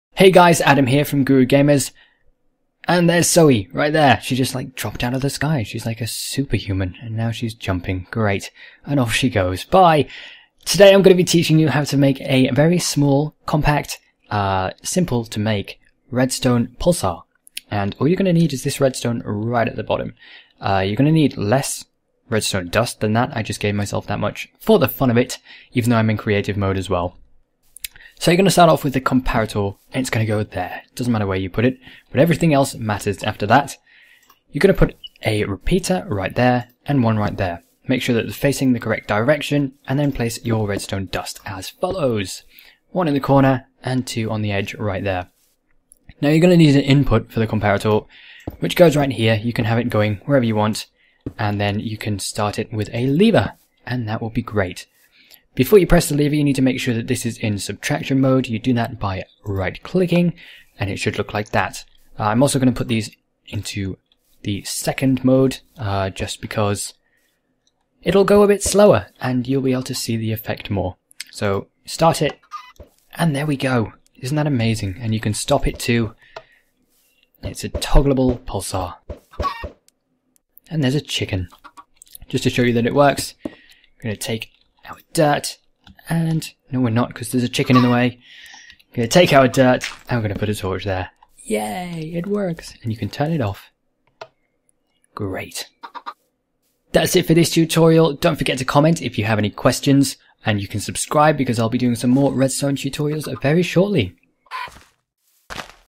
[REDSTONE] Compact toggleable redstone clock/pulsar
A very small and neat toggleable redstone pulsar. Very quick and simple! I will be making more tutorials on pulsars and other redstone circuits soon.